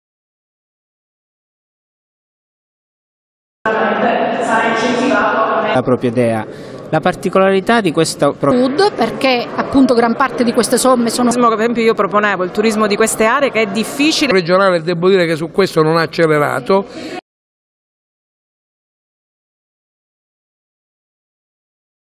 Officine per il mezzogiorno, Resto al Sud
Interviste a On. Maria Iacono , Arch. Toni Cellura, Dott.ssa Cleo Li calzi e On. Giovanni Panepinto. Lunedi ampio servizio